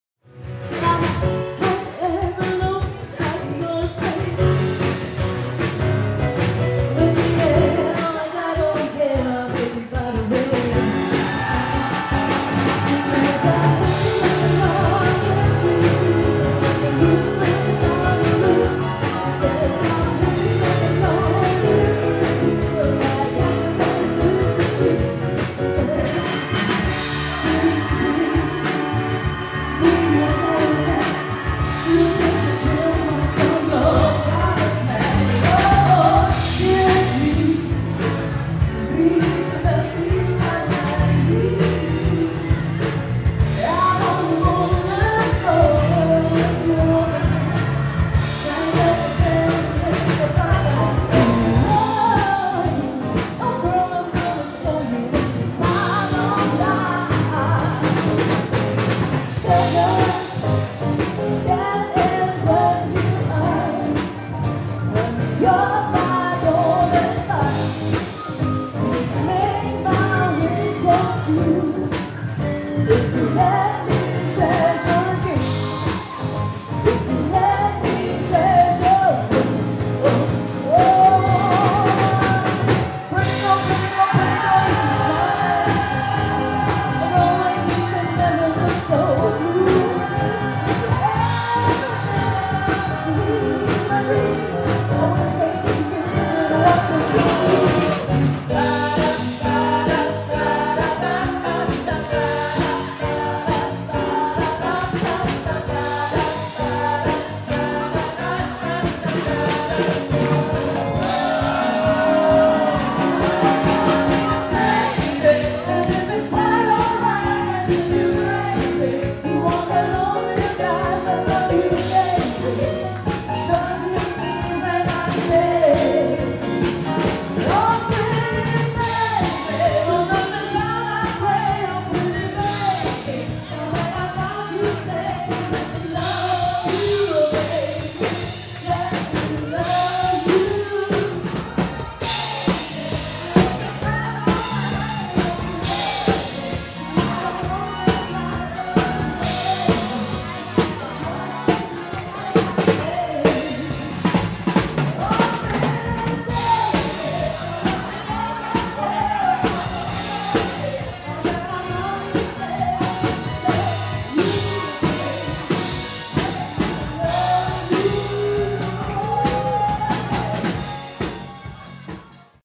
Rachel Yeoh (live performances)
A short preview to Rachel's live performance with band, Wedding in Charlie.